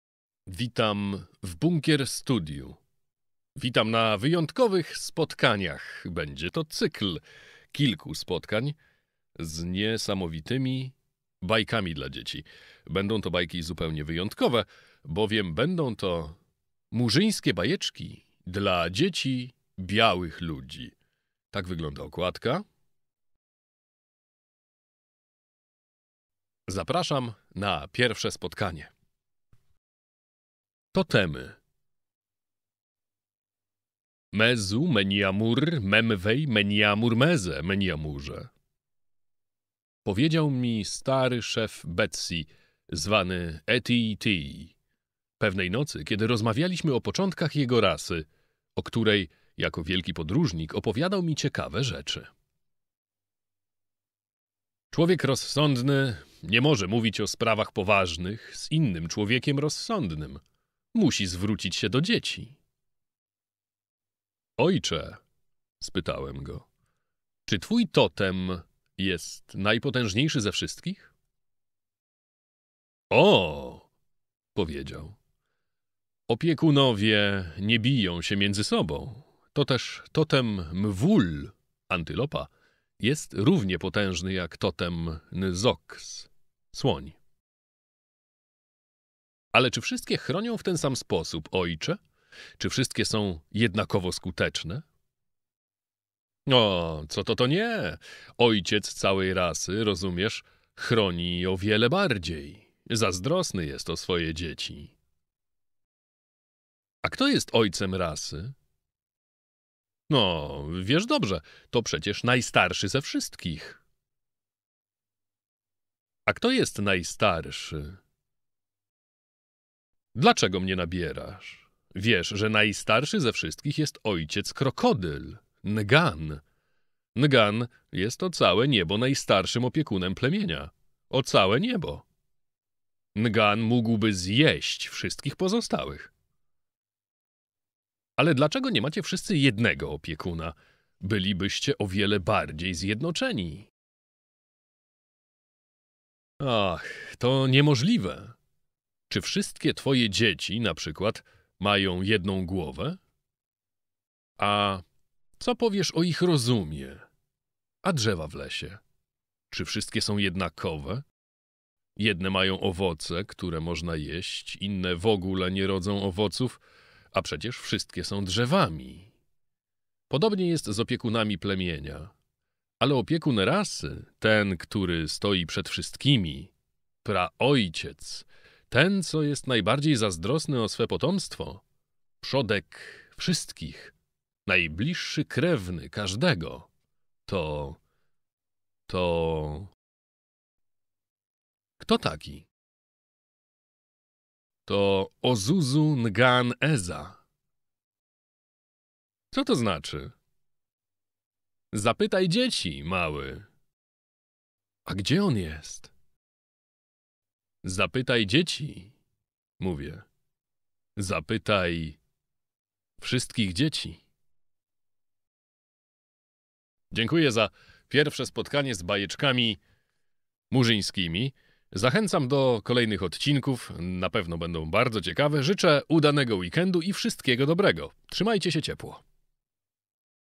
Totemy - z cyklu "Bajki z Bunkierstudia"
Pomyślałem sobie, że dobrze byłoby nagrać coś dla najmłodszych, choć przecież bajki mają charakter uniwersalny i z całą pewnością nie tylko dzieci znajdą w nich coś ciekawego, inspirującego lub odkrywczego.
Zapraszam na pierwszy odcinek serii :)